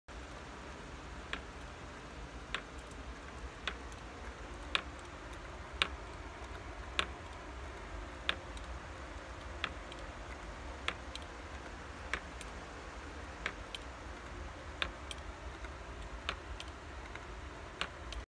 Whirligig fisherman
All oak construction,stainless fittings, 1/8 steel rod shaft threaded 6-32 spinning on ball bearings